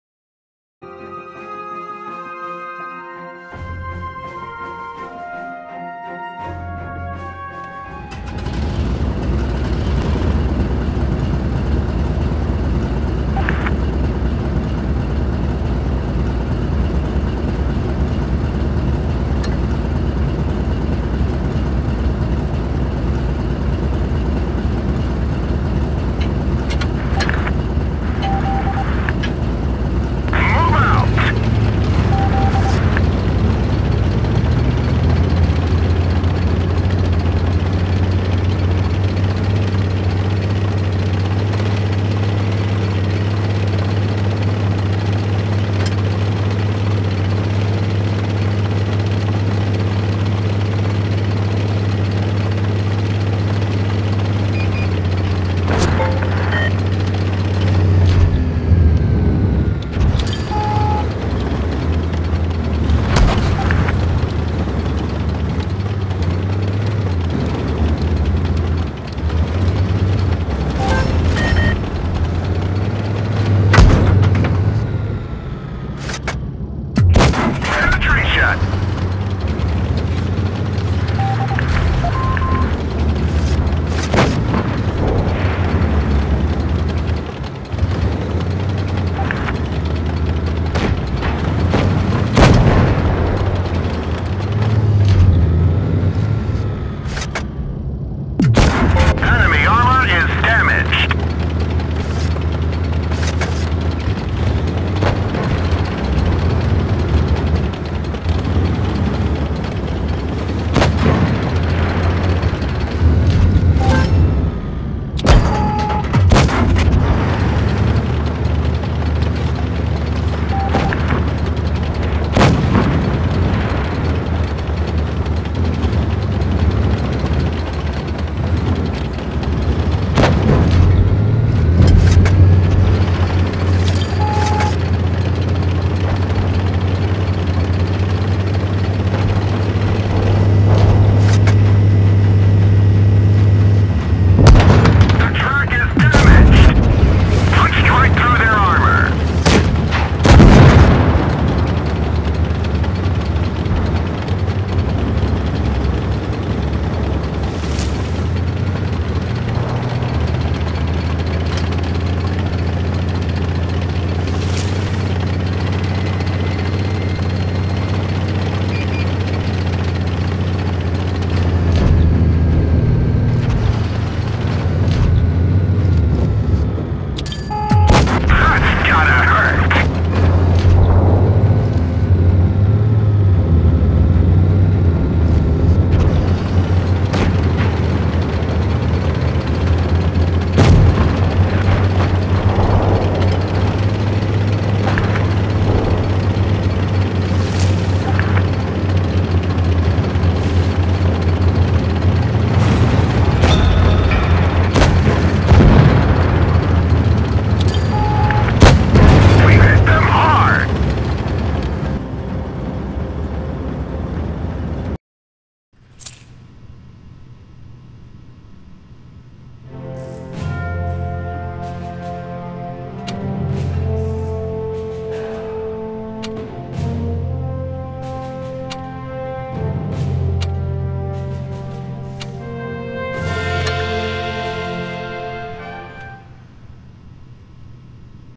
Kpz 70 plays Supremacy!
Quite good Kpz 70 game on Black Goldville in Supremacy mode, showing off its capabilities.